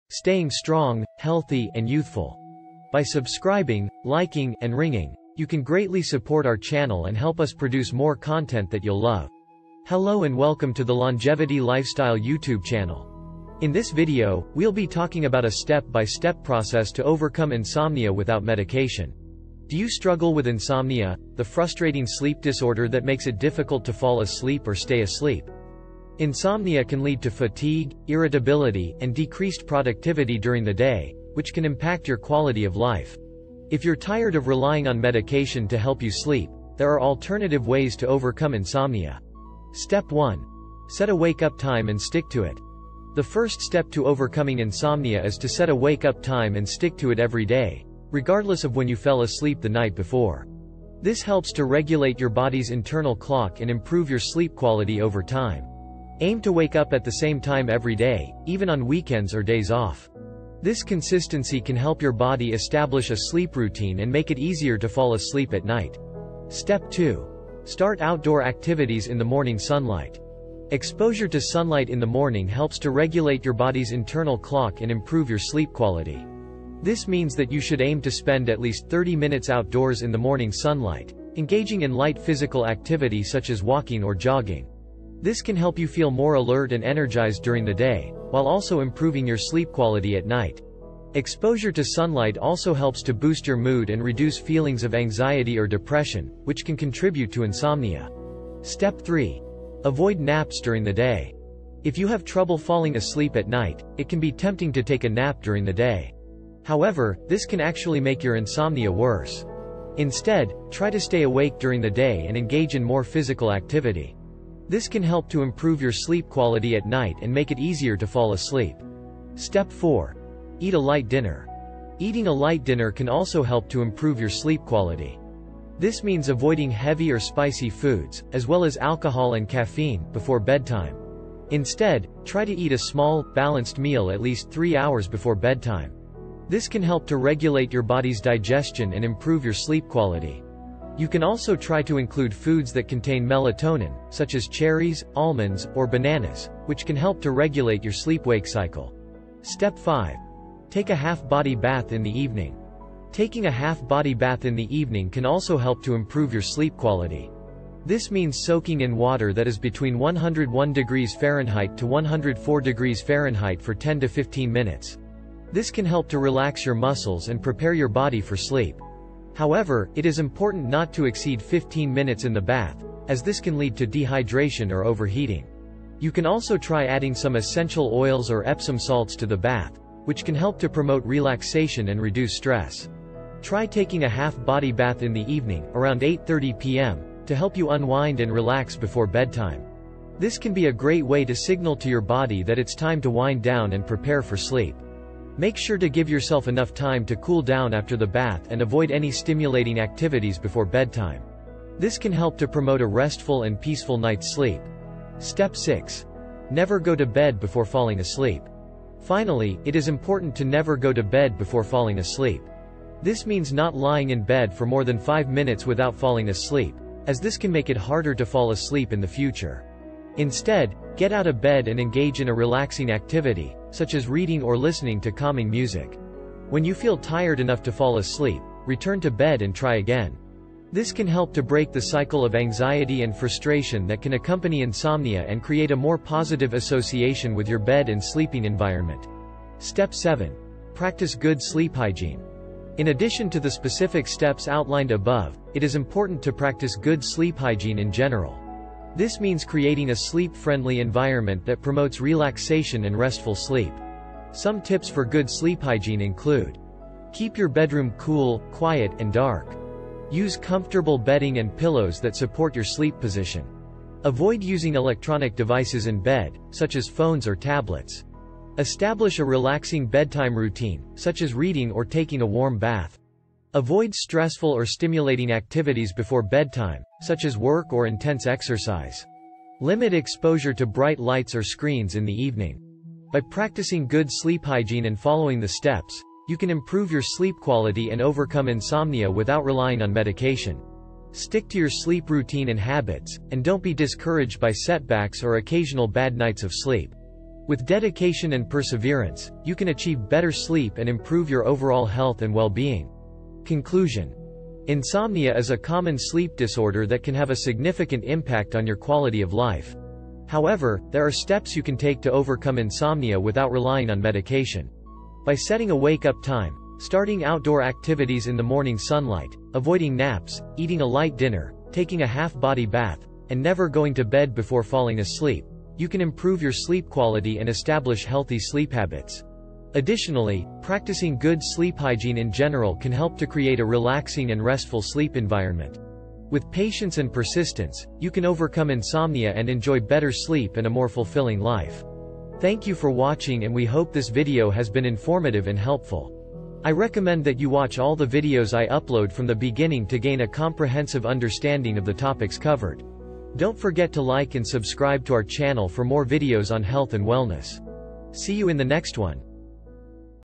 Overcoming Insomnia | A Step-by-Step Guide to Better Sleep Without Medication
This video provides a step-by-step guide on how to overcome insomnia without medication, including setting a consistent wake-up time, getting morning sunlight, avoiding naps, eating a light dinner, taking a half-body bath, never going to bed before falling asleep, and practicing good sleep hygiene.

Step 1: Set a Wake-Up Time and Stick to It
Step 2: Start Outdoor Activities in the Morning Sunlight
Step 3: Avoid Naps During the Day
Step 4: Eat a Light Dinner
Step 5: Take a Half-Body Bath in the Evening
Step 6: Never Go to Bed Before Falling Asleep
Step 7: Practice Good Sleep Hygiene

Conclusion
Insomnia is a common sleep disorder that can have a significant impact on your quality of life. However, there are steps you can take to overcome insomnia without relying on medication. By setting a wake-up time, starting outdoor activities in the morning sunlight, avoiding naps, eating a light dinner, taking a half-body bath, and never going to bed before falling asleep, you can improve your sleep quality and establish healthy sleep habits. Additionally, practicing good sleep hygiene in general can help to create a relaxing and restful sleep environment. With patience and persistence, you can overcome insomnia and enjoy better sleep and a more fulfilling life.